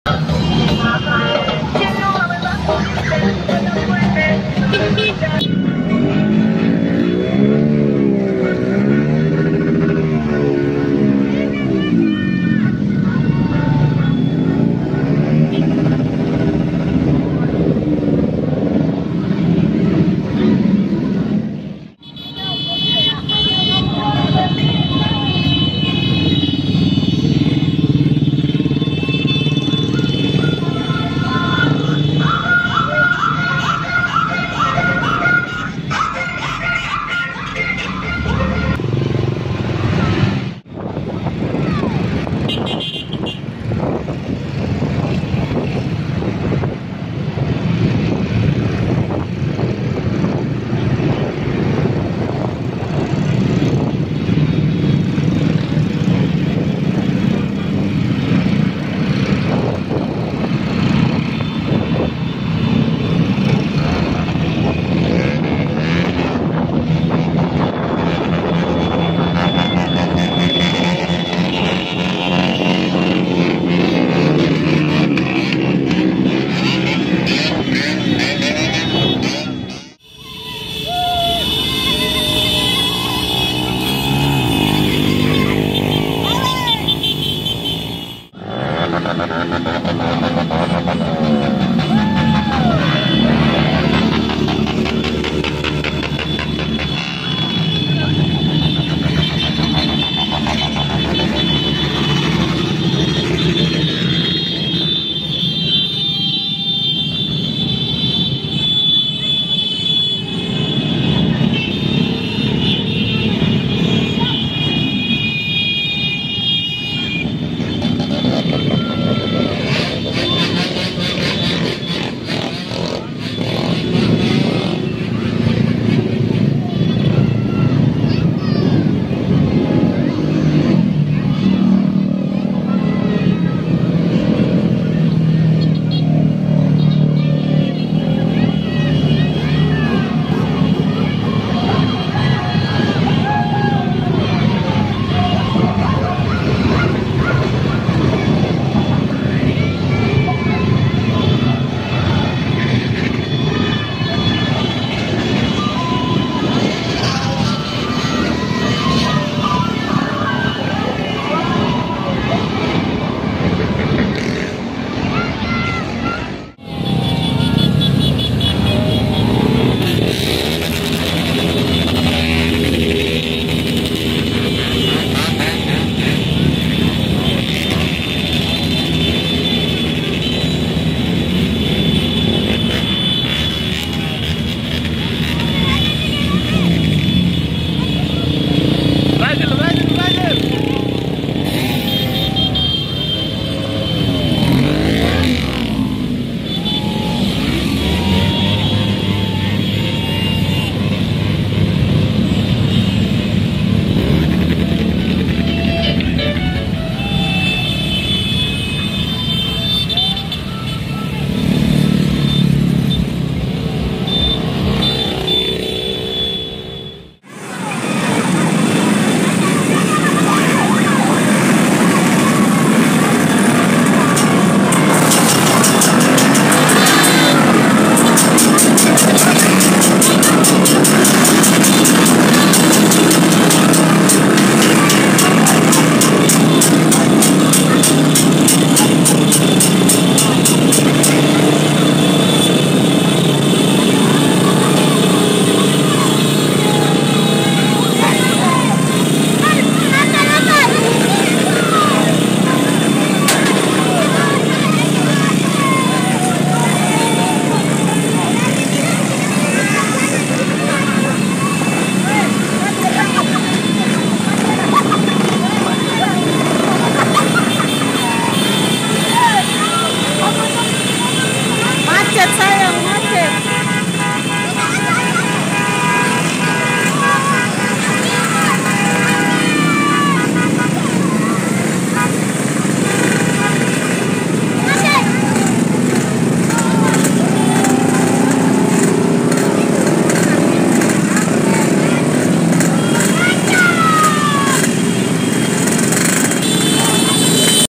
Piala Dunia 2022 || Pawai Fans Berat 5 Negara Besar di Sorong Papua Barat || Terakhir Langsung Macet
Terimaksih Sudah klik video ini.
Di video kali ini saya akan mengajak anda melihat keseruan para fans Berat Negara Negara Besar Timnas  peserta Piala Dunia 2022 Qatar dalam menyambut Piala Dunia 20 november 2022 nanti nya.

Di Sorong Papua Barat sudah mulai Pawai Besar besaran, para fans Berat dari berbagai negara.

ini lah 5 fans Berat Negara Besar di Kota Sorong Papua Barat.

tonton video ini sampai akhir.
yg terakhir Langsung Macet.